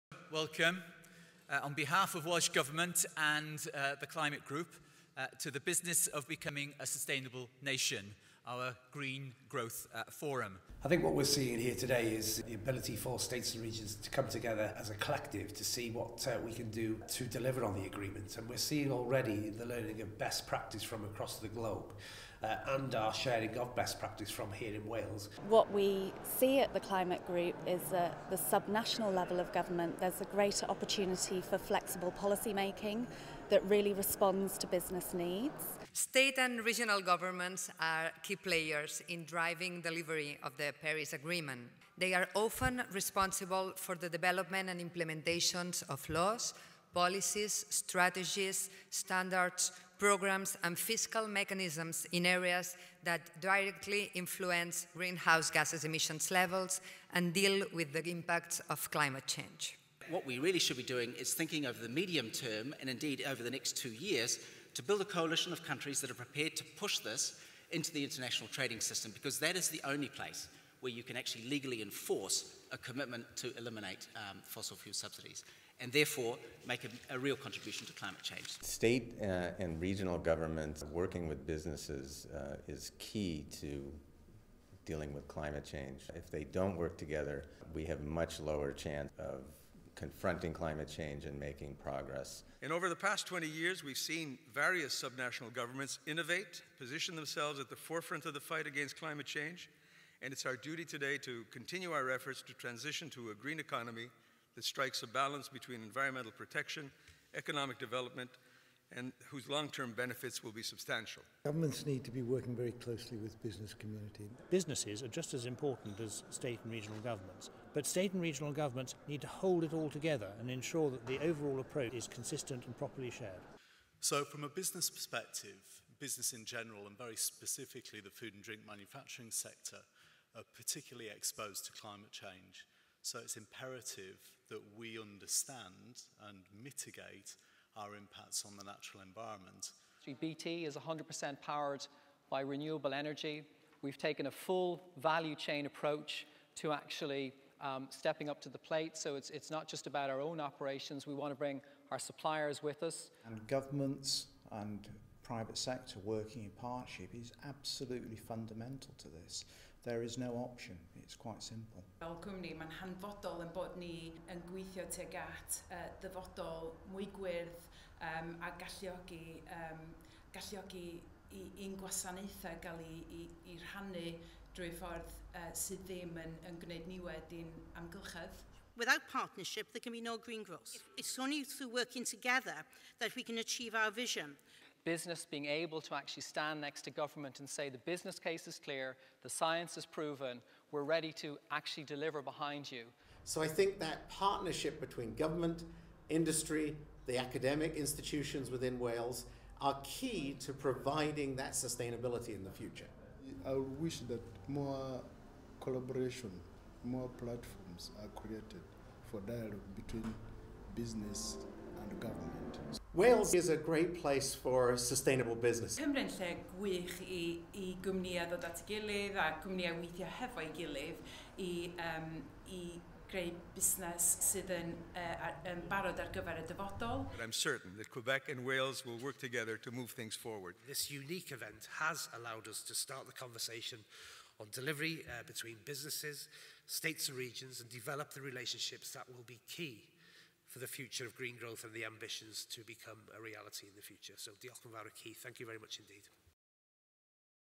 Fforwm Twf Gwyrdd Rhyngwladol 2016 / International Green Growth Forum 2016
Fforwm Twf Gwyrdd Rhyngwladol, Caerdydd 2016 - Y busnes o fod yn genedl gynaliadwy.

Daeth siaradwyr a mynychwyr o wahanol wladwriaethau a rhanbarthau i’r digwyddiad, Roedd hefyd sawl arweinydd busnes a staff llywodraethol allweddol  o’r DU yna. Dyma grynodeb o’r diwrnod

International Green Growth Forum, Cardiff  2016 - The business of becoming a sustainable nation.

The event included speakers and attendees from various states and regions. There were also key business leaders and government staff from across the UK. Here is a summary of the Day